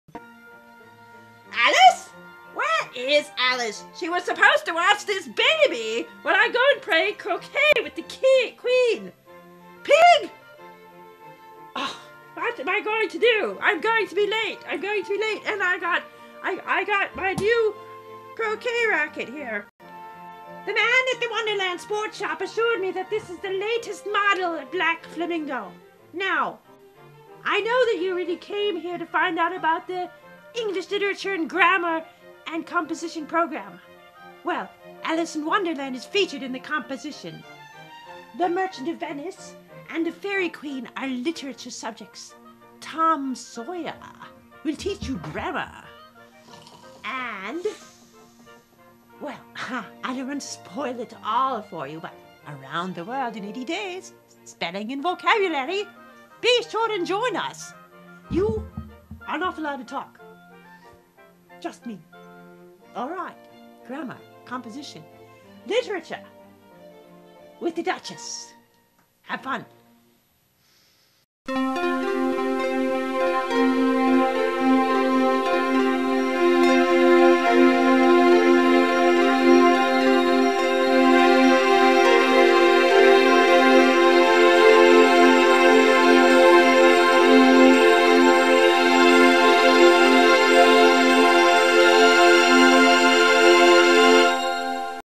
English Skills Teaser
English Skills 84 pages plus .pdf files on CD.
Recommended for upper elementary through High School. Grammar from Tom Sawyer. Composition from Alice in Wonderland. Spelling and Vocabulary from Around the World in 80 Days. Review questions from Animal Farm. Mythology studies that teach literary terms and compare true Scriptural accounts with parallel myths. The allegory of the Faerie Queene. Study of a special condensed version of Shakespeare's Merchant of Venice, plus production tips and guidelines for analyzing what you read, watch and hear with a critical, scriptural eye. Book and teacher's edition CD with .pdf files containing answer keys and a production copy of the Merchant of Venice.
The data CD also includes a number of pictures of costumed dolls representing the Faerie Queene characters as well as many still images from the Faerie Queene Video.